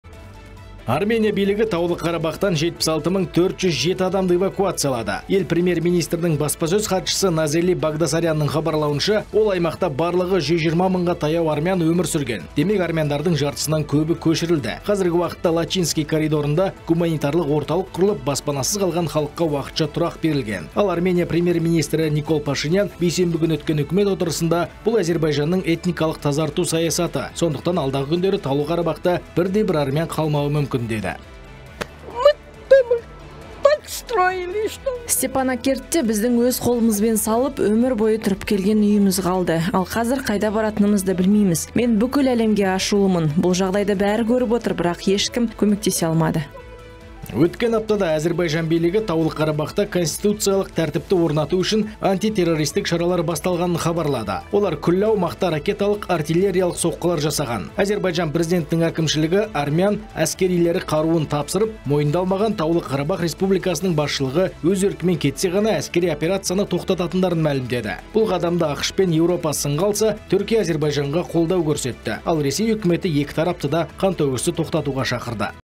Армения билігі Таулы Қарабахтағы этникалық армяндарды эвакуциялай бастады
Армения билігі  Таулы Қарабахтан 76 мың 407 адамды эвакуациялады. Ел премьер министрінің баспасөз хатшысы Назели Багдасарянның хабарлауынша, ол аймақта барлығы 120 мыңға таяу армян өмір сүрген, демек армяндардың жартысынан көбі көшірілді. Қазігі уақытта Лачинский коридорында гуманитарлық орталық құрылып, баспанасыз қалған халыққа уақытша тұрақ берілген. Ал Армения премьер министрі Никол Пашинян, бейсенбі күні өткен үкімет отырысында бұл Әзербайжанның этникалық тазарту саясаты. Сондықтан алдағы күндері Таулы Қарабахта бірде бір армян қалмауы мүмкін деді.